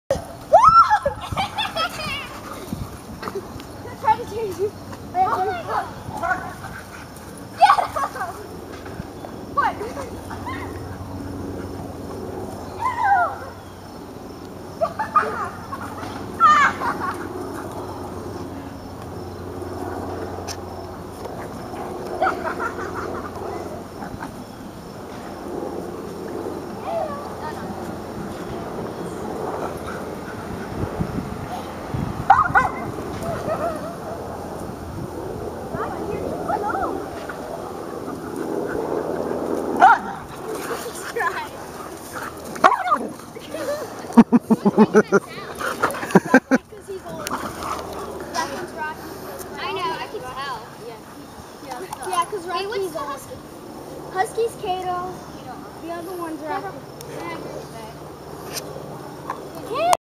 Kids playing with dogs
Took the dogs and Jordan to our "Pet Cemetery" to run around. Two of J's friends were there and wanted to play with them.